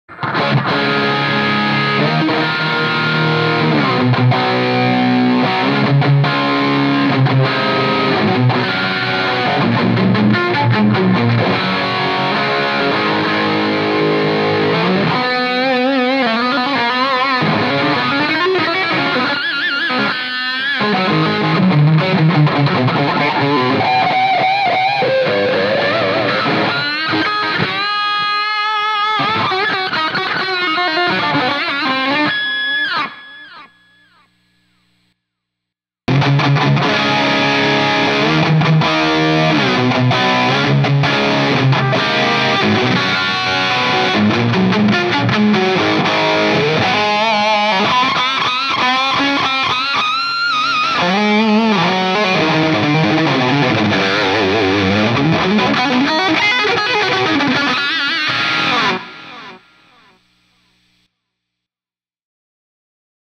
Celestion 25 watt UK made Greenbacks Vs. The new Celestion 65 watt Creambacks
I audio comparison of a UK made Celestion 25 watt greenbacks vs the new 65 watt creamback speakers. Close mic'd using a Shure Beta 58 on the outside edge of the voice coil. Speakers are loaded in old marshall cabs. Amp being used is a RJS 68-100 Plexi style amp (EVH Mod) Pictures in video are generic pictures, not actual pictures of the speakers being tested...